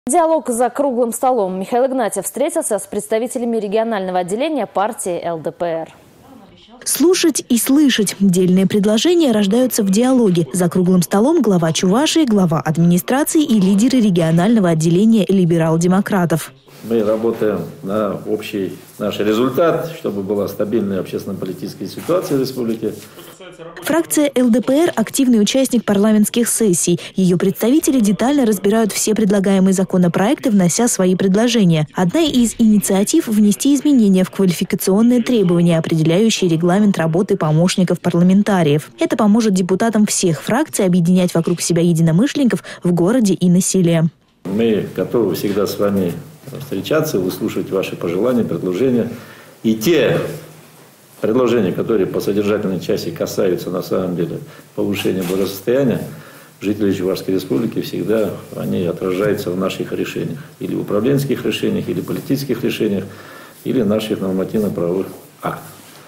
Диалог за круглым столом.
Михаил Игнатьев встретился с представителями регионального отделения партии ЛДПР.